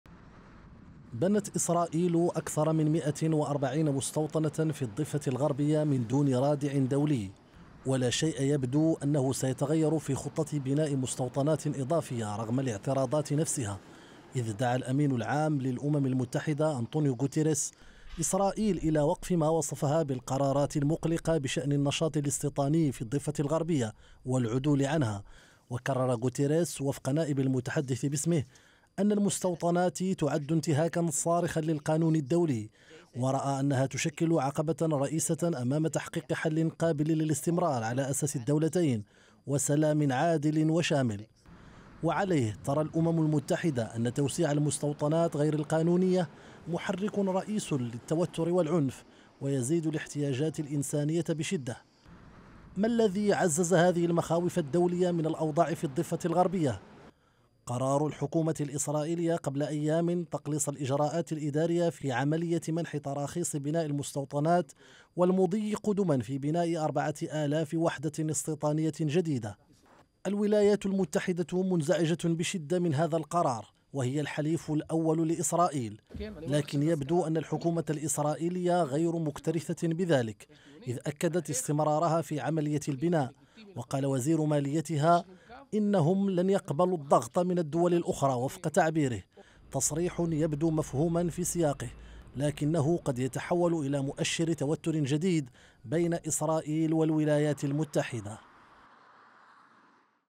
المستوطنات.. هل تؤجج التوتر بين أميركا وإسرائيل؟
حثّ الأمين العام للأمم المتحدة إسرائيل على وقف القرارات المُقلقة بشأن المستوطنات في الضفة_الغربية فيما أعربت أميركا عن انزعاجها الشديد من هذه القرارات.
فهل تستجيب إسرائيل؟